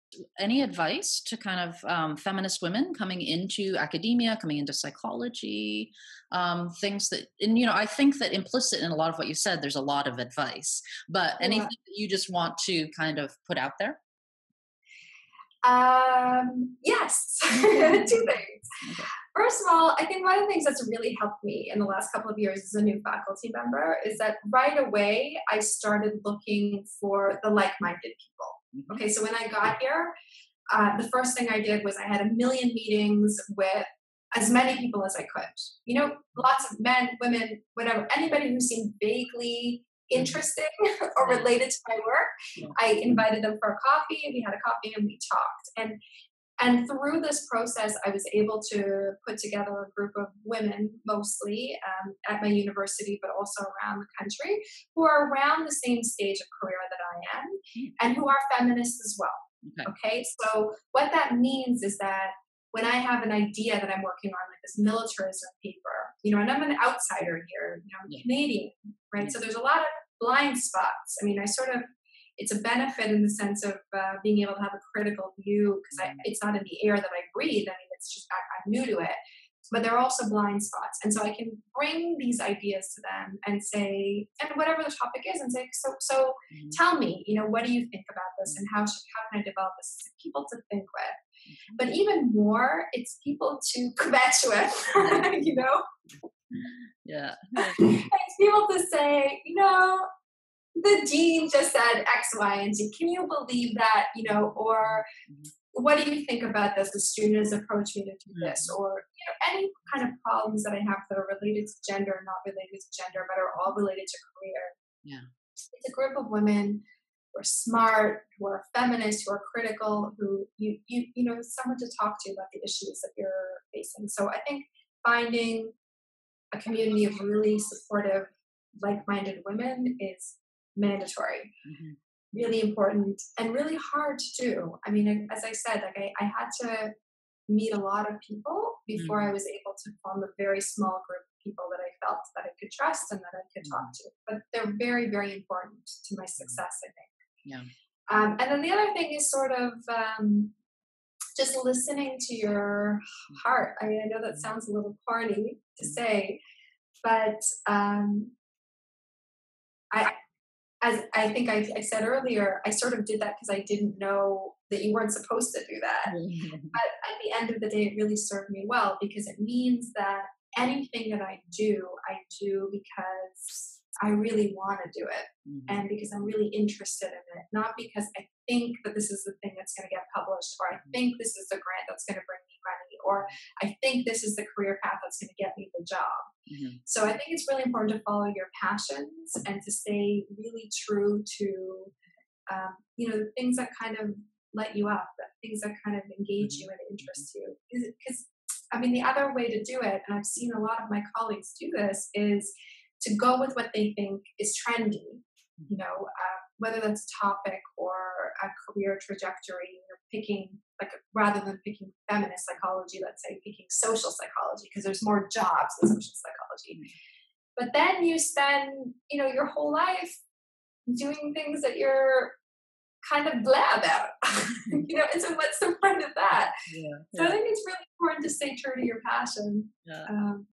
PFV Interview with Leeat Granek: Advice to Young Feminist Psychologists
Leeat Granek offers advice to young feminist psychologists.

This is an excerpt from one of the many oral history interviews available